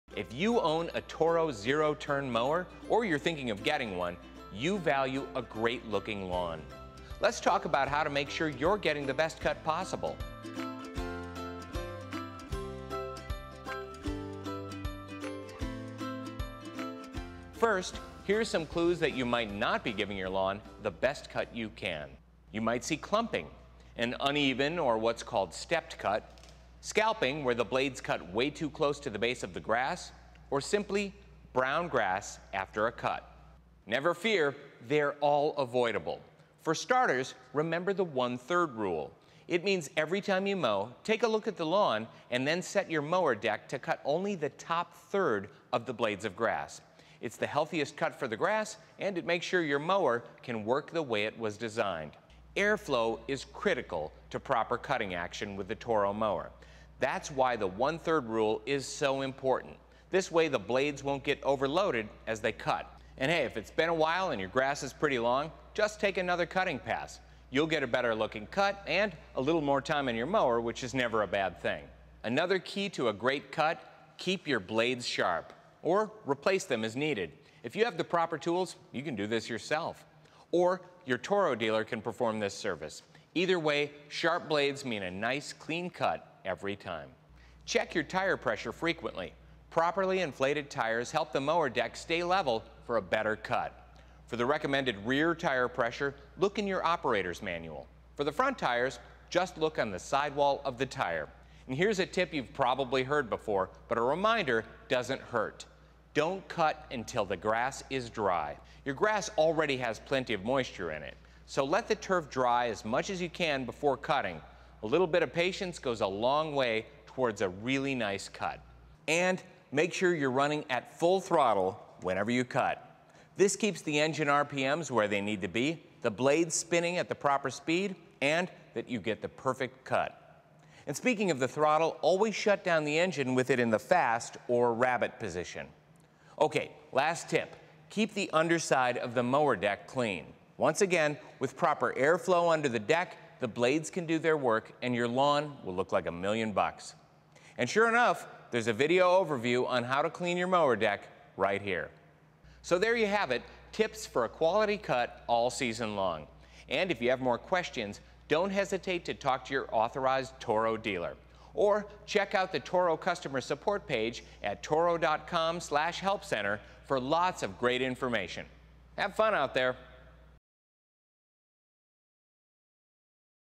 Toro Tips for a Quality Cut
How to get the best quality of cut from your lawn mower. Do you see clumping, uneven grass after you cut or is your mower scalping the grass? Then here are some great tips.

for FAQs, manuals and more.